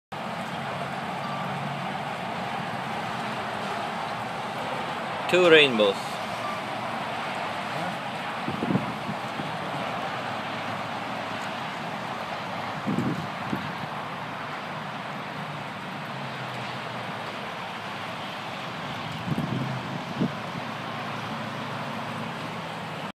Two rainbows march 28 2012
Two rainbows seen at davie Florida